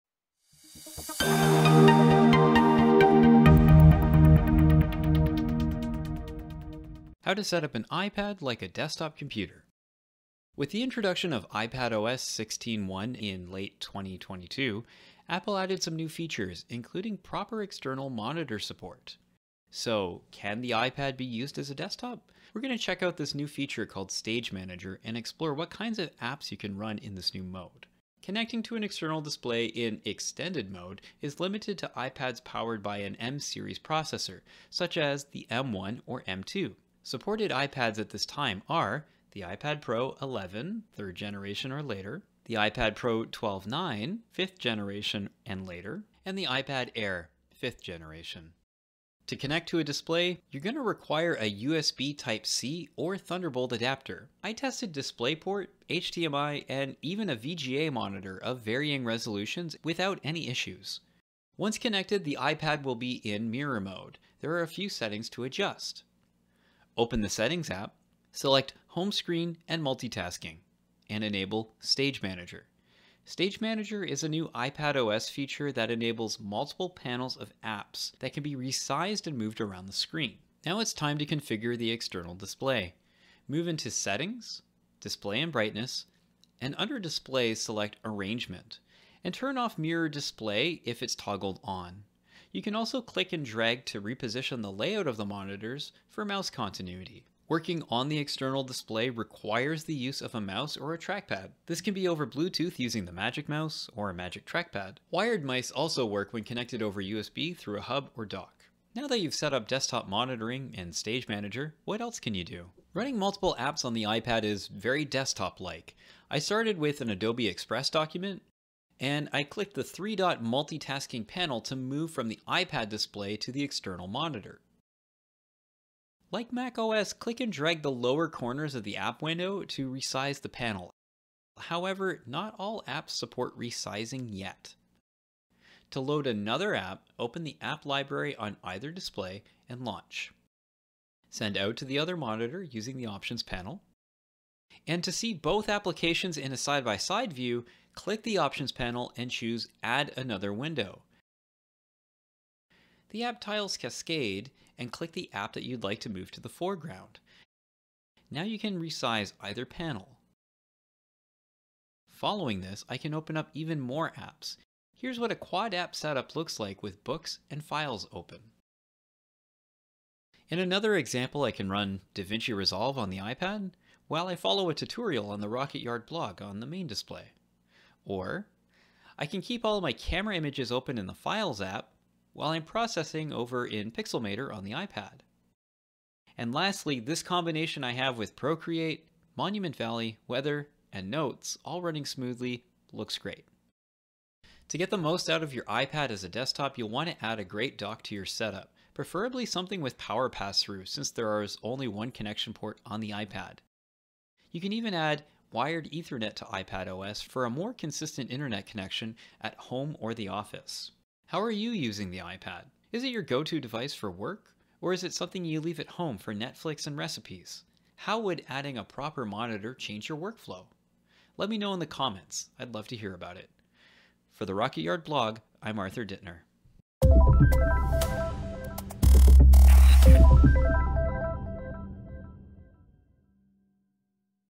How to setup an iPad like a desktop computer
With the release of iPadOS 16.2, iPads powered by an M1 or M2 processor have gotten closer than ever to being able to replace a laptop or desktop computer. In this video, you'll learn about Stage Manager extended display, which allows you to connect your iPad Pro or iPad Air to an external display and run up to 8 windowed apps simultaneously across your iPad's built-in display and the one on your desktop.